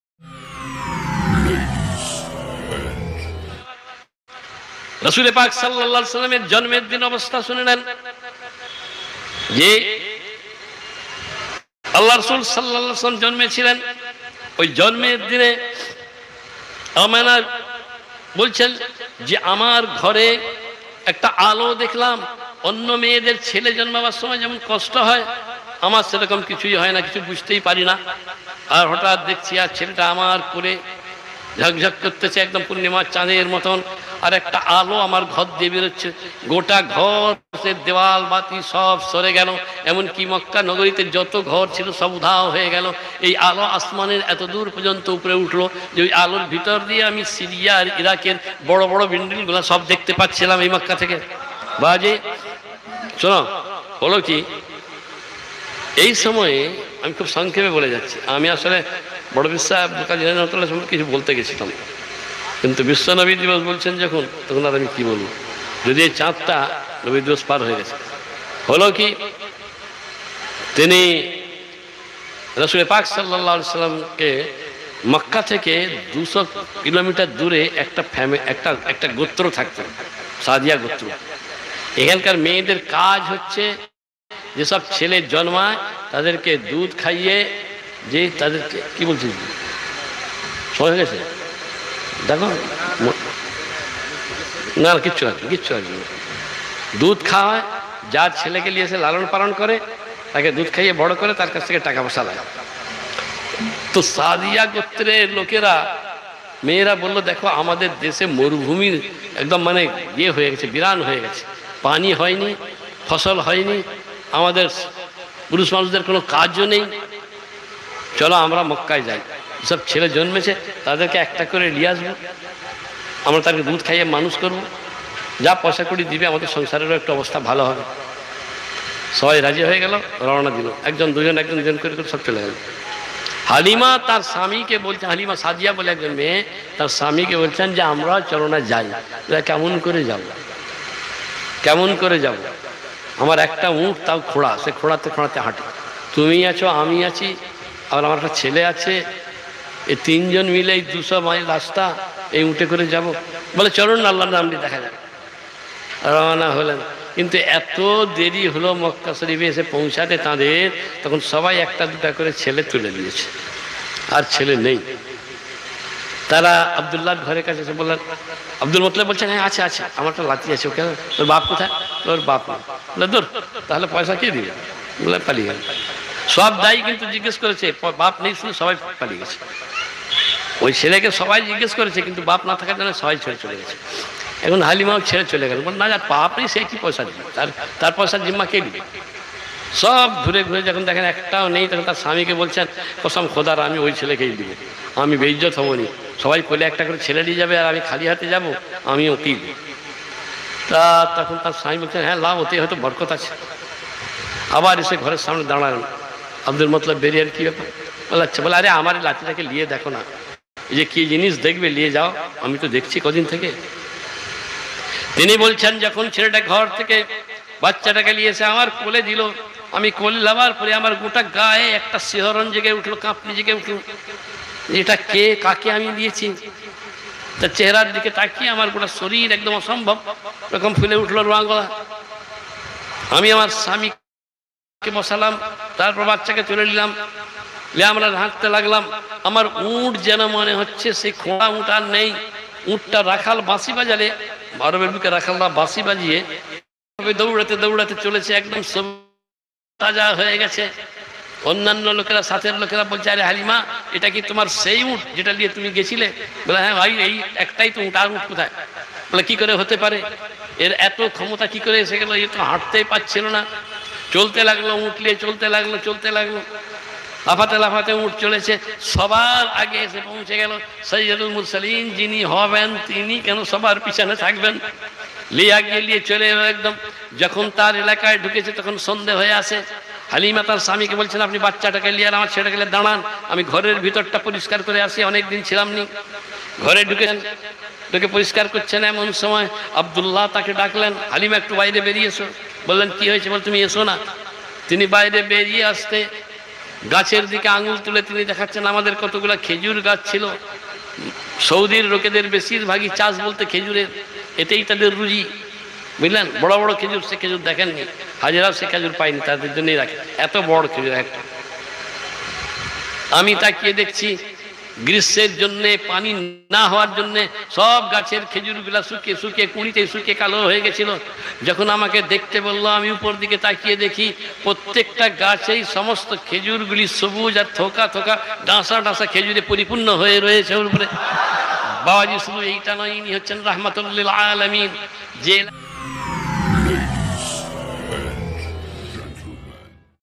নবীজির জন্মের দিনের ঘটনা।। নবীজির জন্মানোর পর কি ঘটেছিল।। পীর আল্লামা সৈয়দ খালেদ হোসাইন সাহেব
সাহাবারা কিভাবে দান করতেন সৈয়দ রুহুল আমিন ভাইজান জালসা